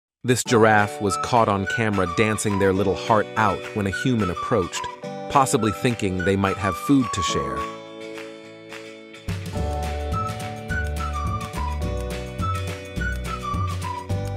Tall Tales of Dance: Giraffe's Unexpected Moves!
Dive into the wild with 'Tall Tales of Dance: Giraffe's Unexpected Moves!' 🦒🕺 Get ready to witness the savannah come alive as our majestic giraffe takes the dance floor by storm. 🌟 From surprising twirls to unexpected footwork, this video is a dance fusion extravaganza that will leave you in awe.

🦒 Join the safari shuffle as the giraffe and its human dance partner create a duo that defies expectations. 🌍 Experience the magic of the wild and the joy of unexpected encounters, all set to a lively soundtrack that will have you grooving along.

🎉 Whether you're a wildlife lover or a dance enthusiast, 'Tall Tales of Dance' guarantees a safari soiree like no other. 🎥 Don't miss out on the laughter, joy, and unforgettable moments that unfold in this dance spectacle. Hit play and let the giraffe's unexpected moves steal the show! 🚀